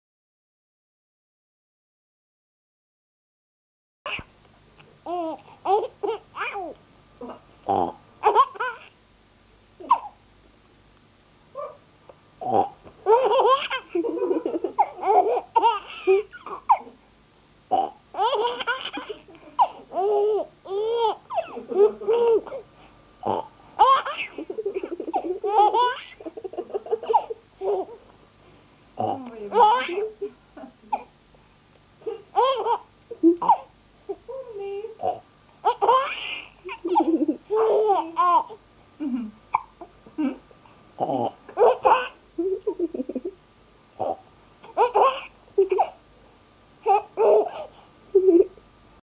Alex, the laughing baby
My son Alex burst out laughing when I make strange sounds. And he gets the hickups.